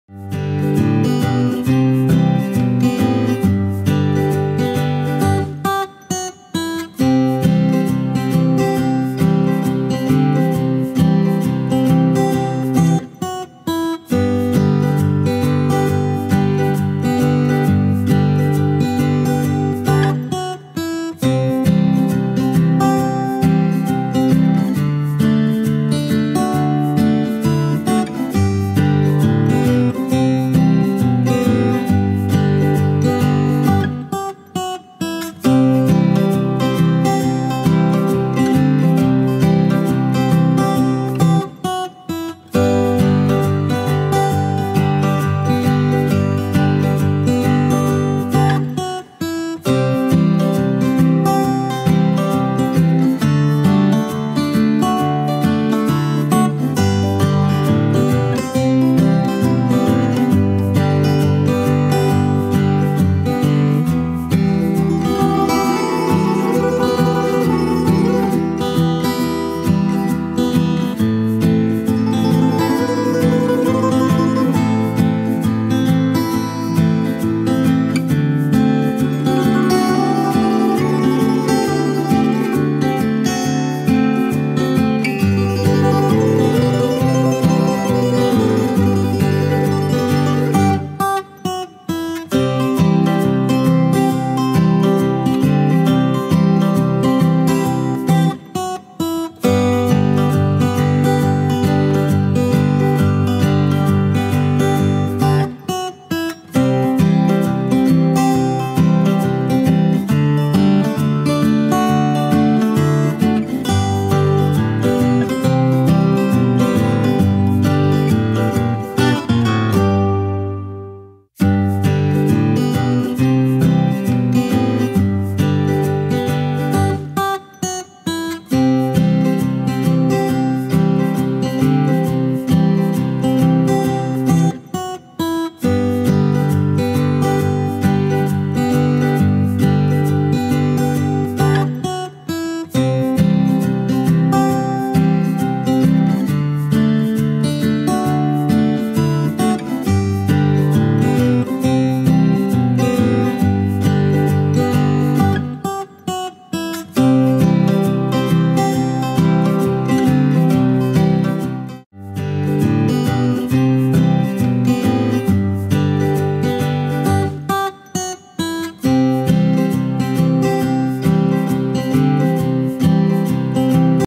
Cost Of Living In Stuttgart, Germany In 2019, Rank 148th In The World
These data are based on 1484 entries in the past 18 months from 140 different contributors. Please help us to update the data, thank you.